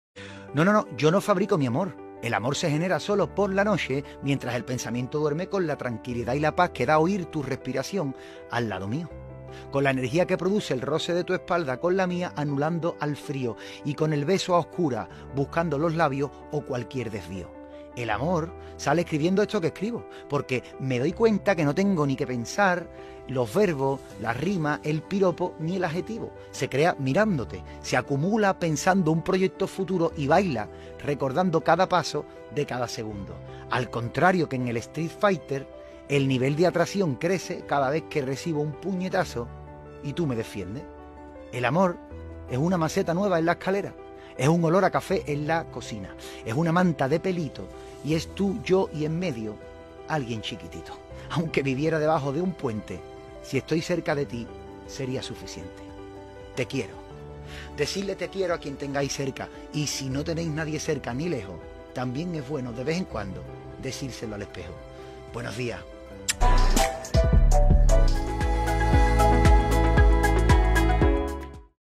Fabiola Martínez comparte un precioso vídeo con su hijo Kike como protagonista
La ex de Bertín está volcada en su fundación y en los cuidados de su hijo Kike, a quien le dedica un tierno vídeo